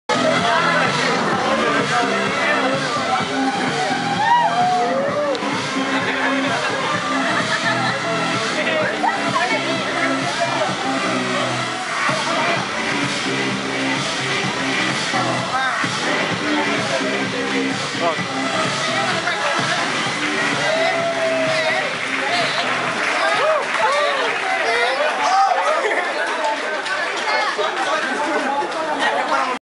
Partenón Acapulco 2012 - Dance with the Cosplay
Los cosplayers del Partenón echándose unos bailecitos. XD!!